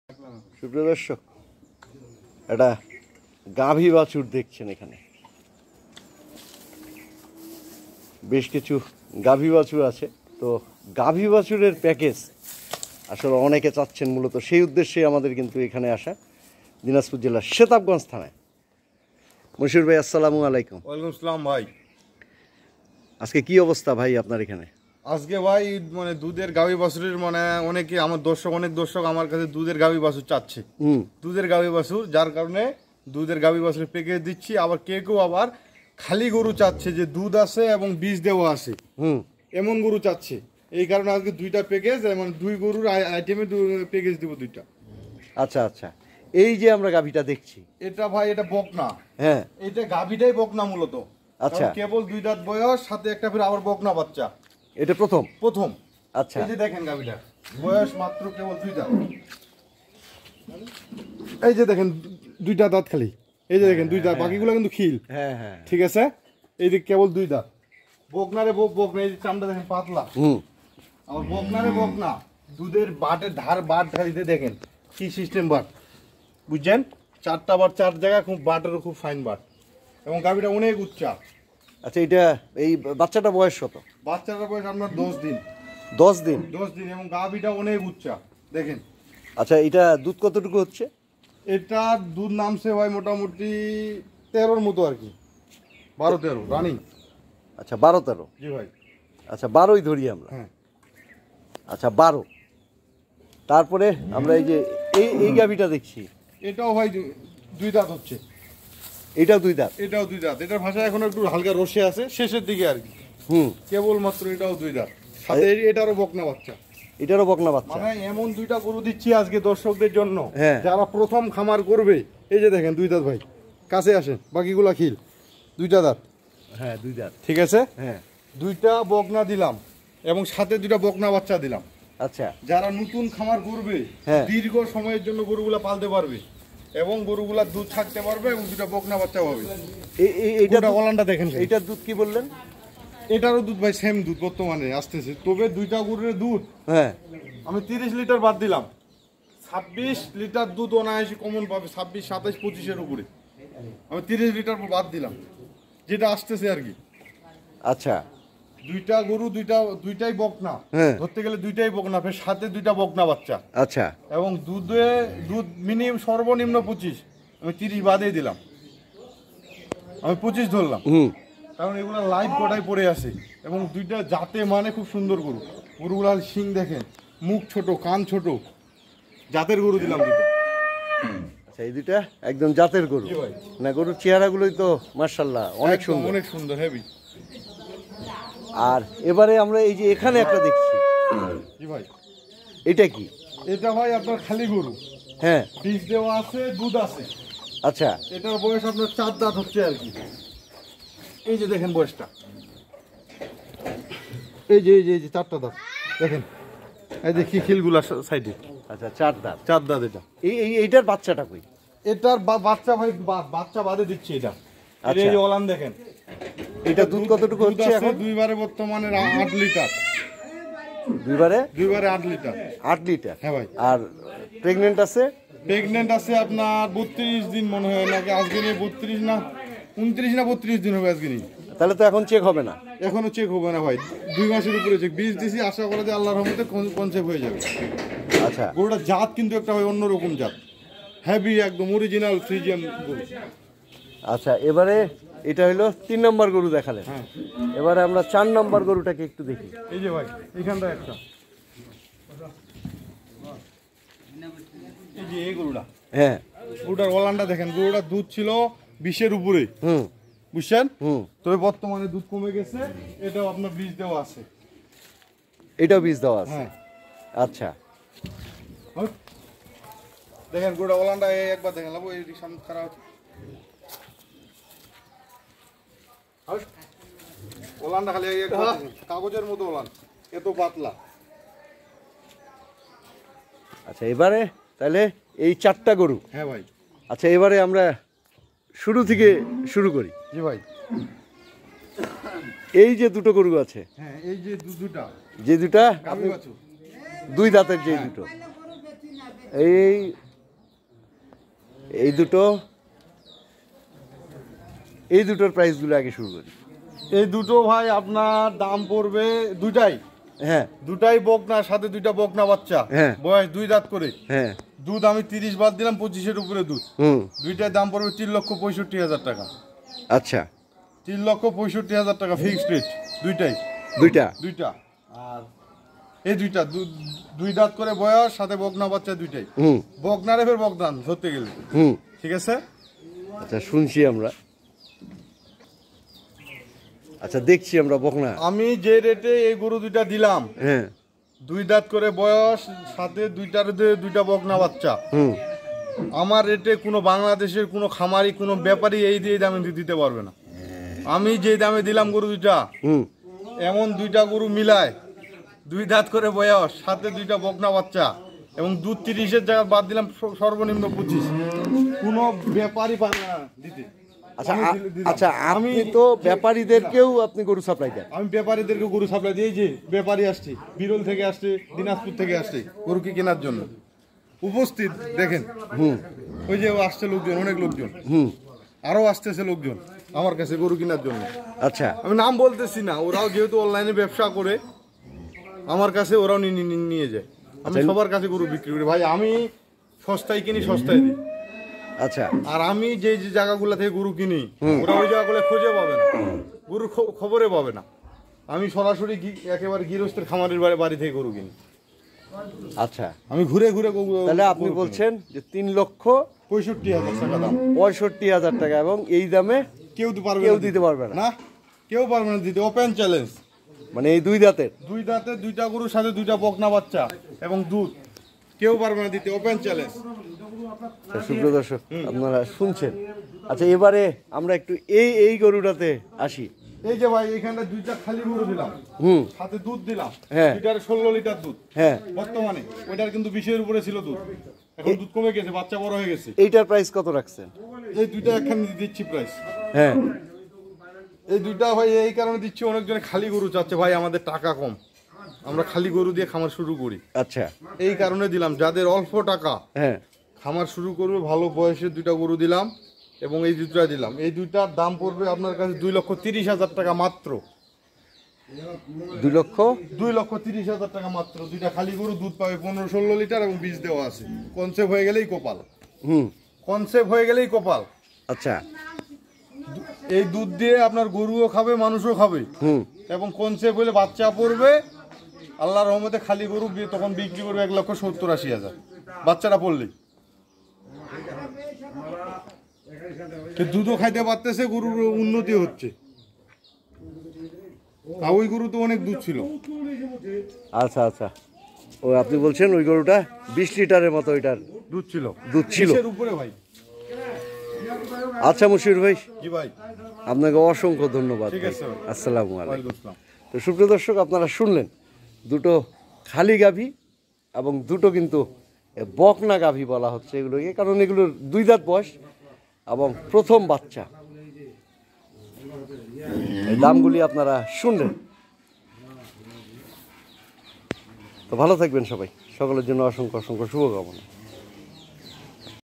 কমদামের চ্যালেঞ্জ দিয়ে গাভীর প্যাকেজ || স্বল্প দামে দুই দাঁতের গাভী বকনা বাছুরসহ দাম জানুন।
মশিউর রহমানের সাথে যোগাযোগ -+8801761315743